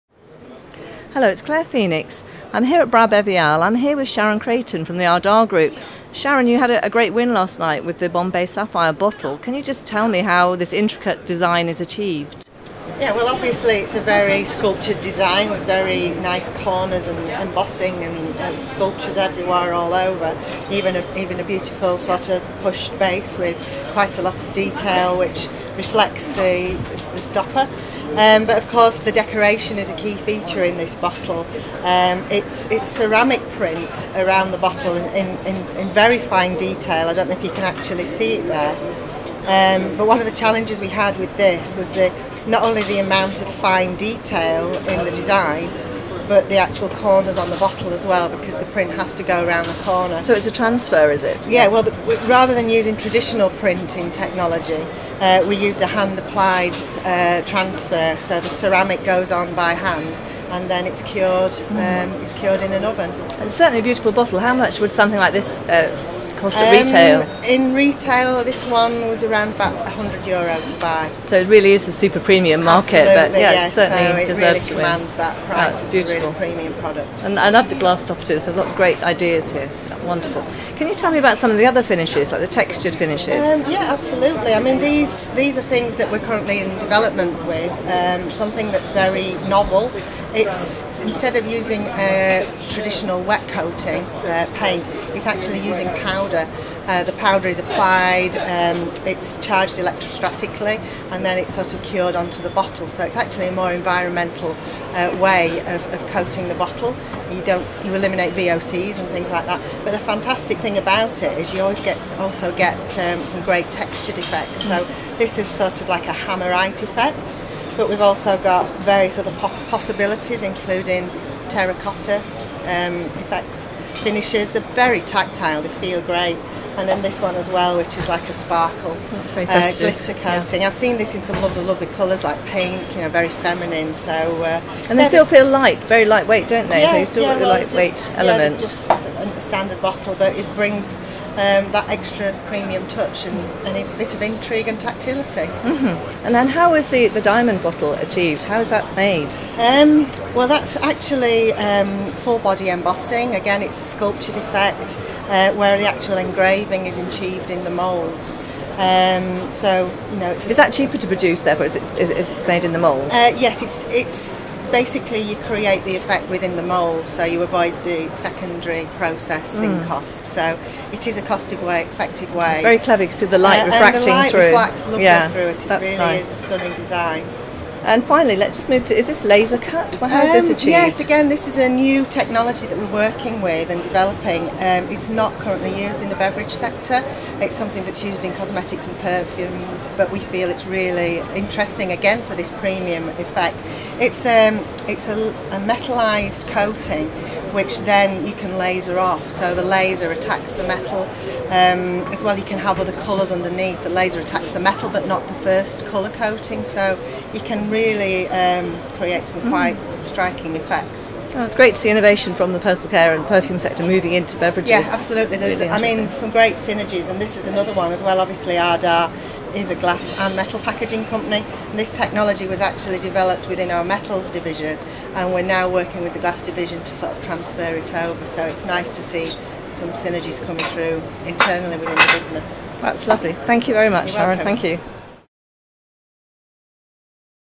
Ardagh Group’s Sharon Crayton talks about the award winning Bombay Sapphire bottle
-- The morning after the presentation of the World Beverage Innovation Awards at BrauBeviale, FoodBev Media’s Claire Phoenix talked to Ardagh Group head of marketing (glass Europe) Sharon Crayton, about the company’s win in the Best bottle in glass category – and other innovations in glass.